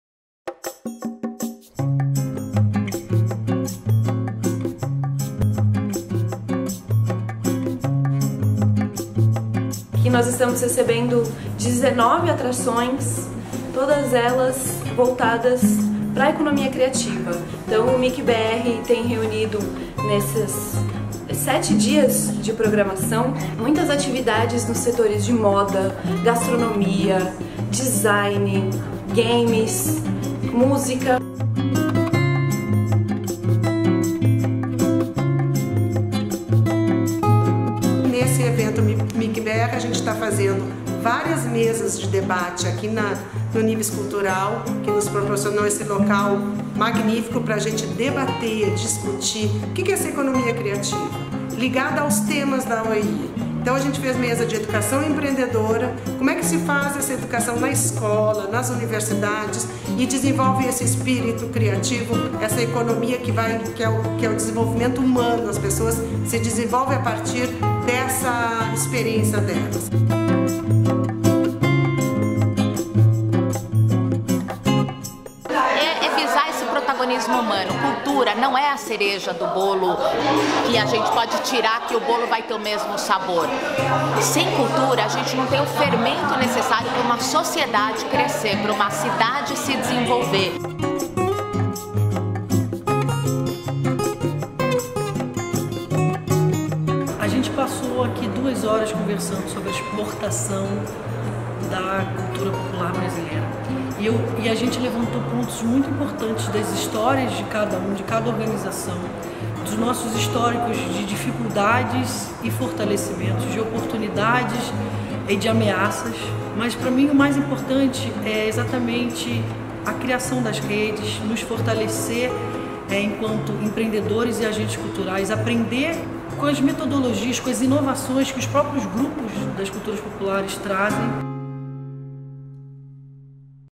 MicBR - Mercado das Indústrias Criativas do Brasil
A iniciativa abrange dez áreas da produção cultural: artes cênicas, artes visuais, animação e jogos eletrônicos, audiovisual, design, editorial, gastronomia, moda, música e museus e patrimônio. Empreendedores brasileiros e de sete países hermanos – Argentina, Chile, Colômbia, Equador, Paraguai, Peru e Uruguai – participam do evento.

 Fizeram parte da programação a série Cozinha Show – aulas de gastronomia comandadas por renomados chefs latino-americanos – e as palestras “Fotografia de Moda e Publicidade na Era de Smartphones, Redes Sociais e Novas Mídias”, com os fotógrafos Miro, Andre Schiliró e Claudia Guimarães, e “Food Truck e Comidas de Rua”, com o jornalista e empreendedor André Fischer.

O MicBR contou com parceria do Itaú Cultural, SESI-SP, SESC-SP, Sebrae, Universidade de São Paulo, Pixel Show, Zupi.co, Japan House São Paulo, secretarias estadual e municipal de Cultura de São Paulo, Anima Mundi, Livraria Cultura, MASP e Instituto Moreira Salles, além da cooperação da Unesco e da Organização dos Estados Ibero-Americanos (OEI) e o apoio de várias entidades representativas de setores criativos da indústria. Confira!